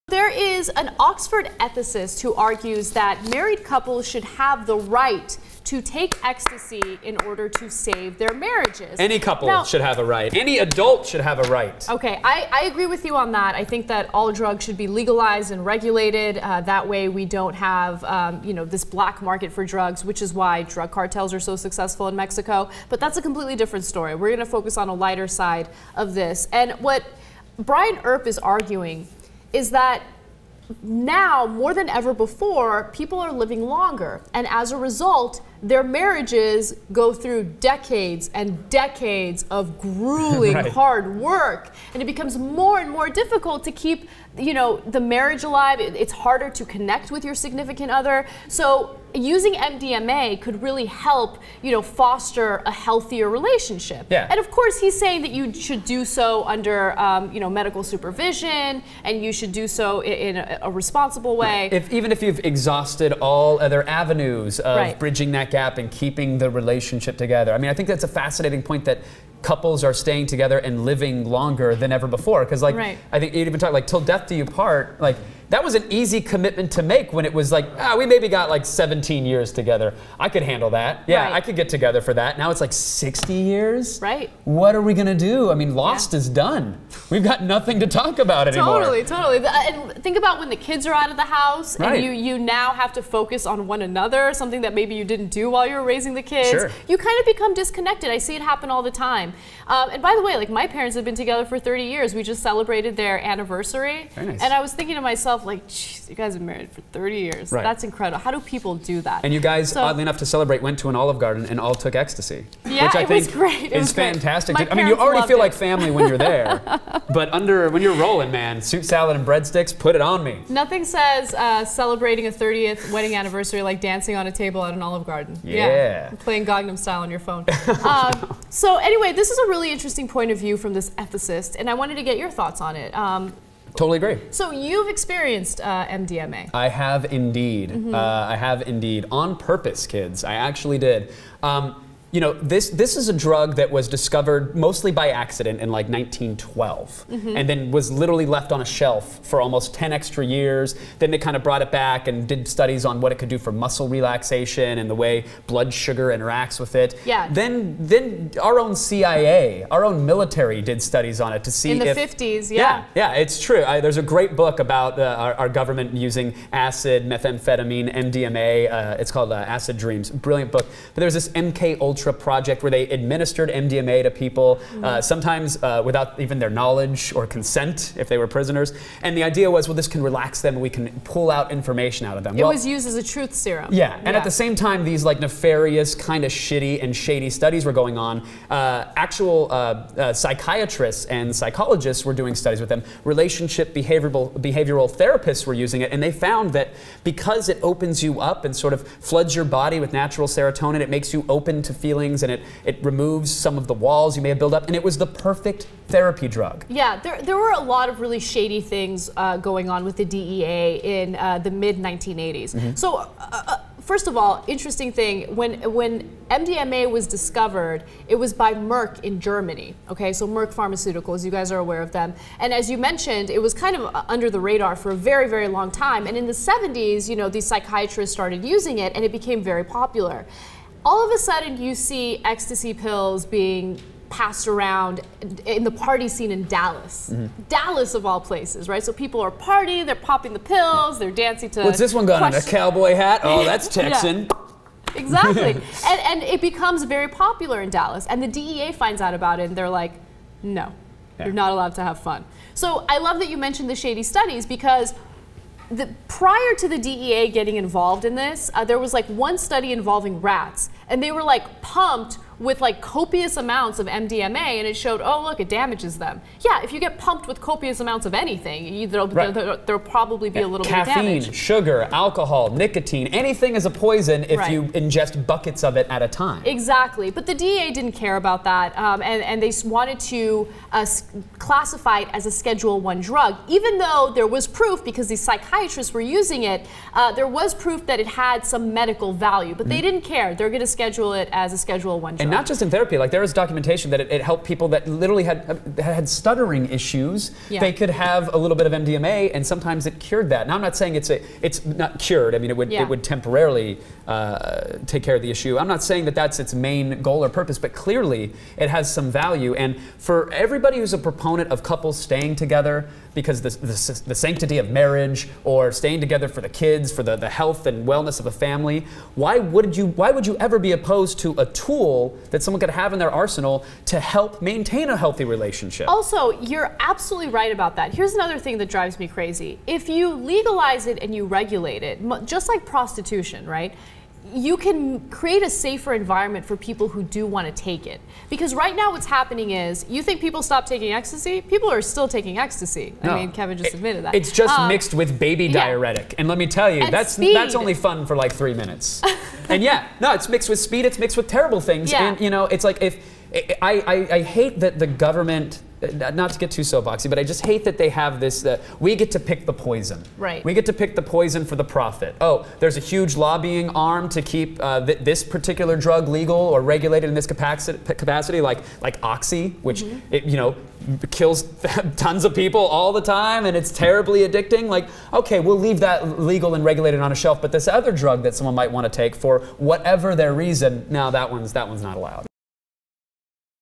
Take Ecstasy to Save Your Marriage?
"The divorce rate is high. Internet dating has  killed romance. And we are all  going to die alone.

But it might not have to be that way, says Oxford ethicist Brian Earp and colleagues Anders Sandberg and Julian Savulescu. Not if couples start taking ecstasy, that is. So-called love-inducing drugs like oxytocin and MDMA could be the answer to a commitment-averse culture."

An Oxford ethicist argues that couples should be allowed to take MDMA, or ecstasy, in order to save marriages. Could the "love-enhancing" drug save romance?  Ana Kasparian and Kevin Pereira (Super Creative) discuss.